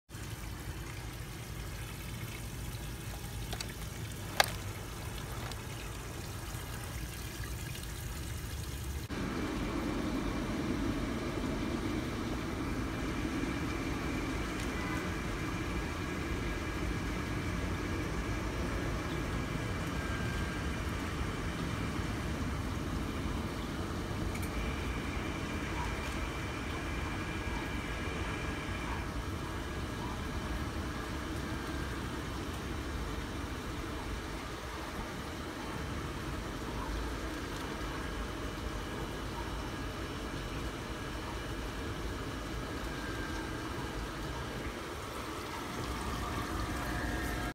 Gemmy 2017 animated Hugging snowman Christmas inflatable
Gemmy 2017 animated hugging snowman inflatable sold for 100$ at "at home" picked this one up 50% off, this one is pretty cool for only 50$ on the 50% off sale. Christmas inflatable hugging snowman